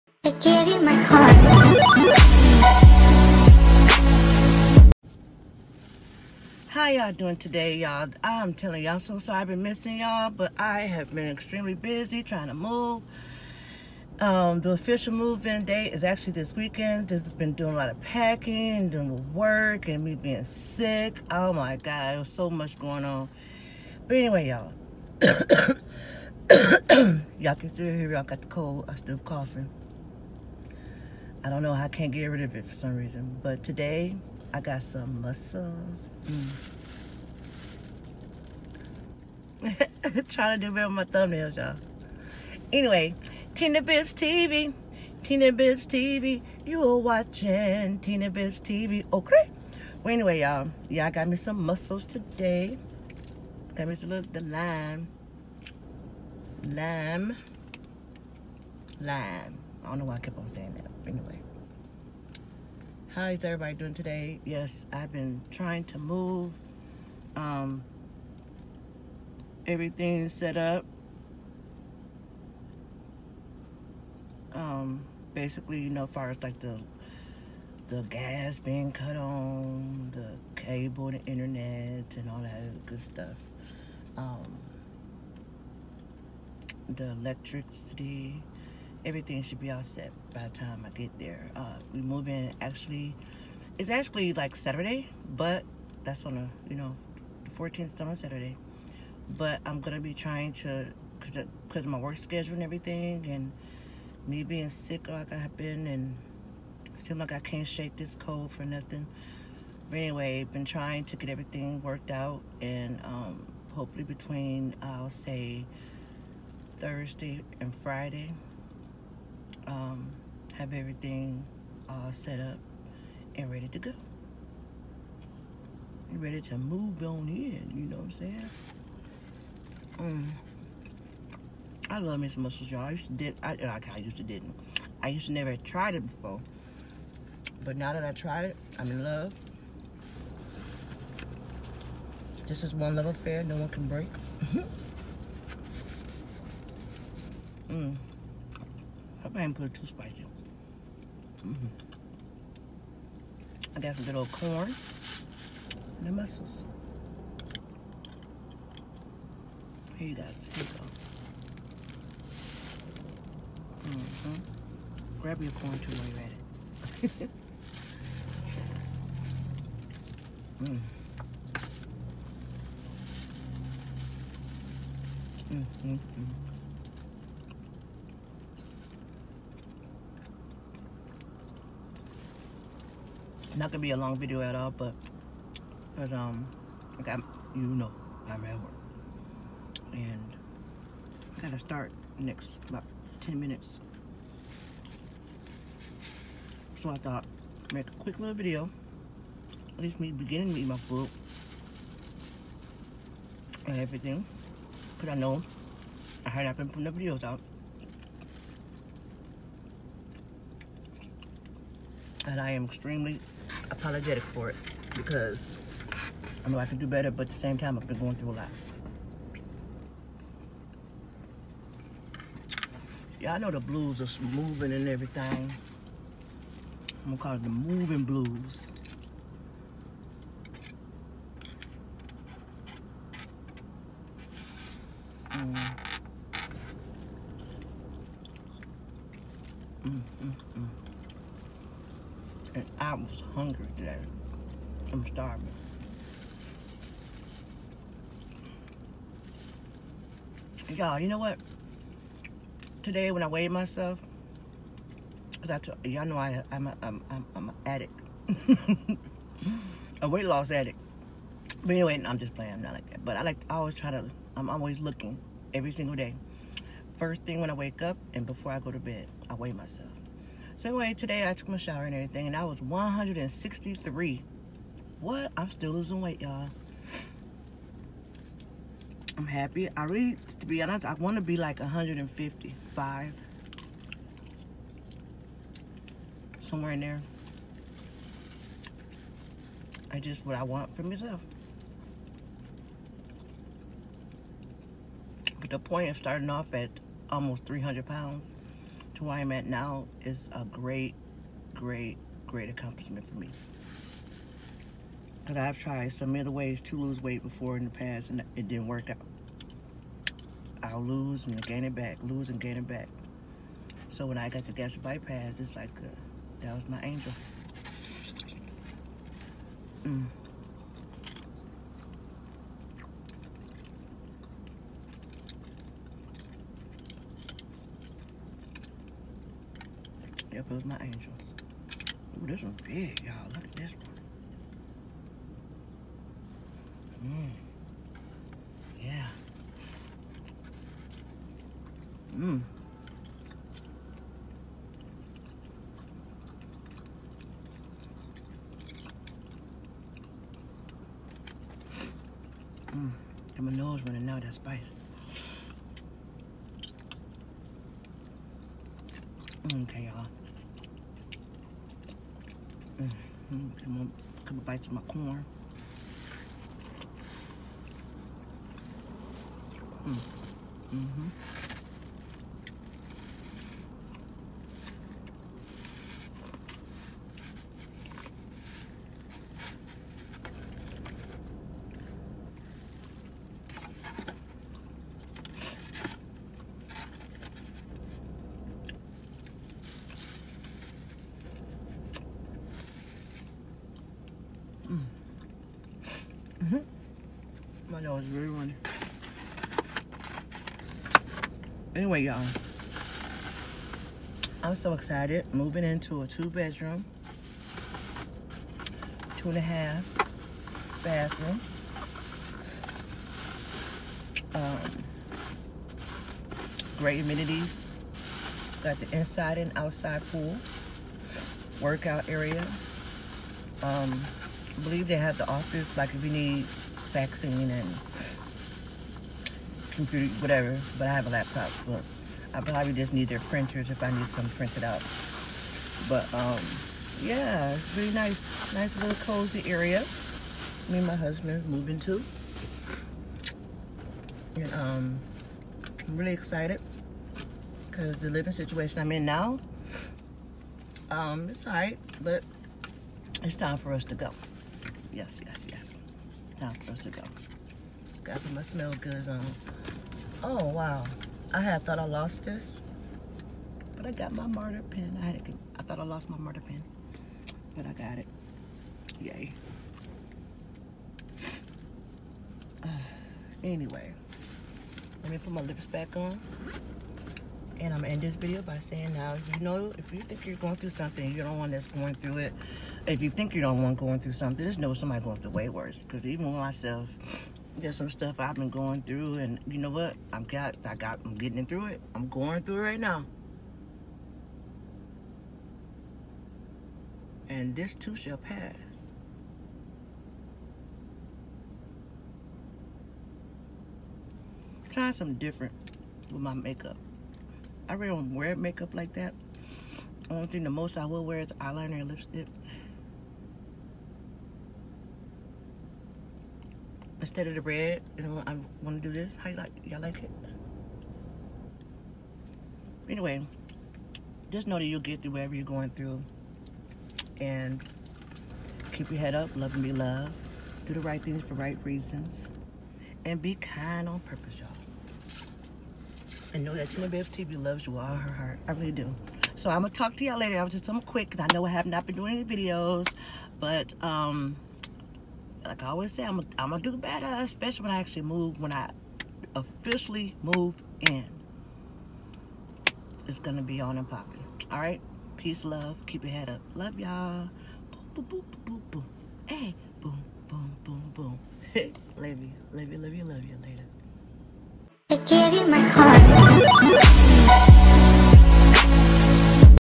Mussels and Corn King Seafood Eating Show | Why I have been Absent
Hey y’all. Hope everyone is enjoying their day. Miss y’all. I’m trying to move and been really busy and on top of that I have a cold. Not Coronavirus. Once I’m situated, I’ll be putting out more content. Thanks for hanging in there for me.

Eating Mussels and Corn from King Seafood.